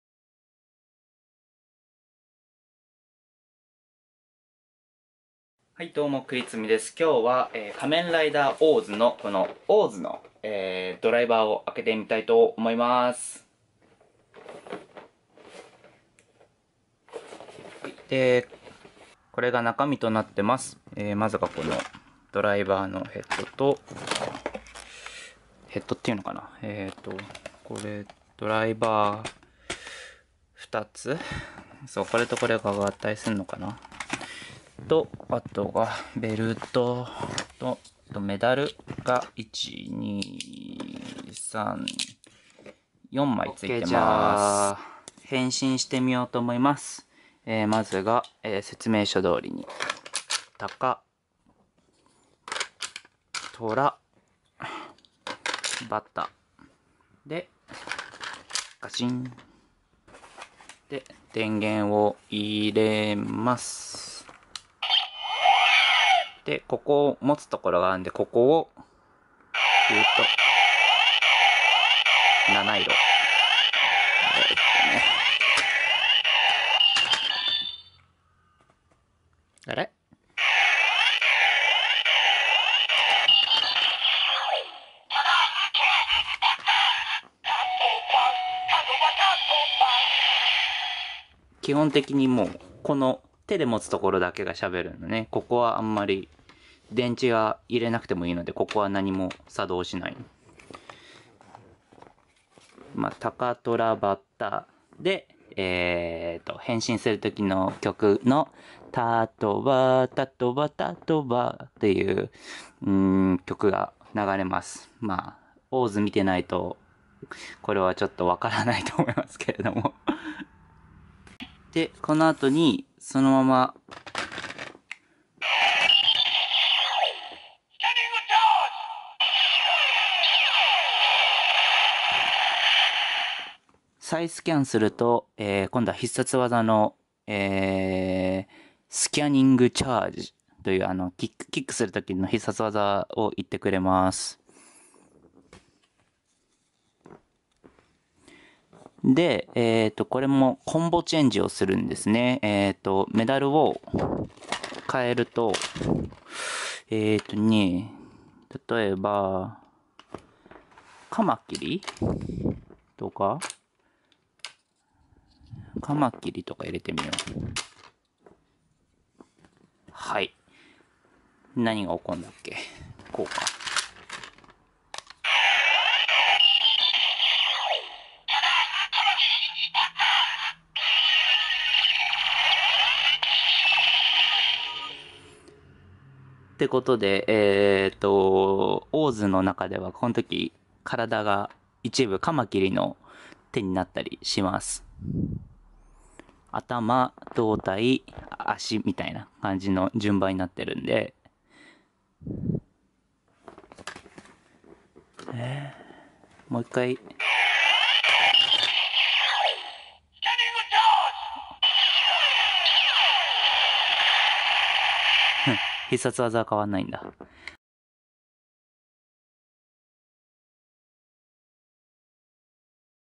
仮面ライダーオーズドライバー/Quick Review of Kamenrider OOO Driver (Eng Sub)
Just a quick review of Bandai Deluxe Kamenrider OOO belt.
 手抜きシリーズで、自分のおもちゃ紹介初めてみた。